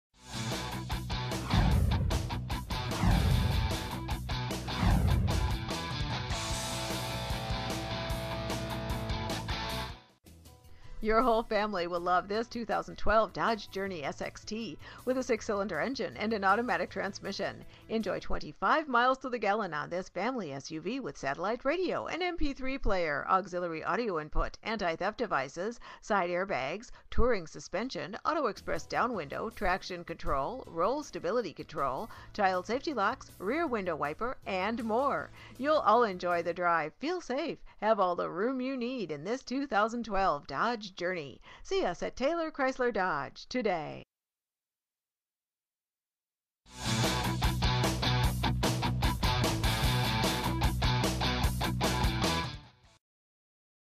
Dodge Journey, Taylor Chrysler Dodge- Bourbonnais, IL 60915
Dodge Journey, Taylor Chrysler Dodge at Taylor Chrysler Dodge, we know you are not just looking out for you. you need a vehicle for your entire family. check out this RED 2012 Dodge Journey, equipped with a 6 Cyl. engine and an automatic transmission with only 14 miles. enjoy an exceptional 25 miles to the gallon on this family SUV with features like satellite radio, auxiliary audio input, touring suspension, on steering wheel audio and cruise controls, Tire Pressure Monitoring System, cargo light, heated outside mirrors, and much more. Enjoy the drive, feel safe, and have peace of mind in this 2012 Dodge Journey. see us at Taylor Chrysler Dodge today!

miles-     14

vin- 3C4PDCBG7CT361456

Taylor Chrysler Dodge

1497 North Route 50

Bourbonnais, Illinois 60915